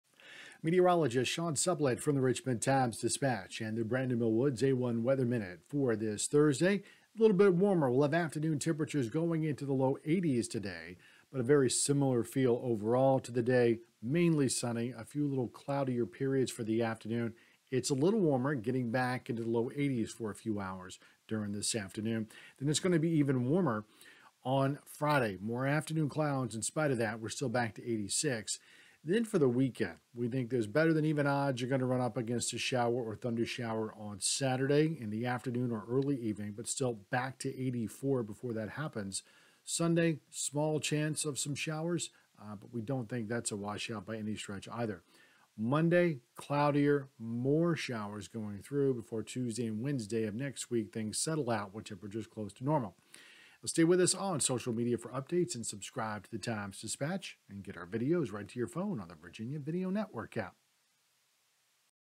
Thursday morning weather video: Warmer weather builds into this weekend
More humid to start the weekend, and be on the lookout for some Saturday showers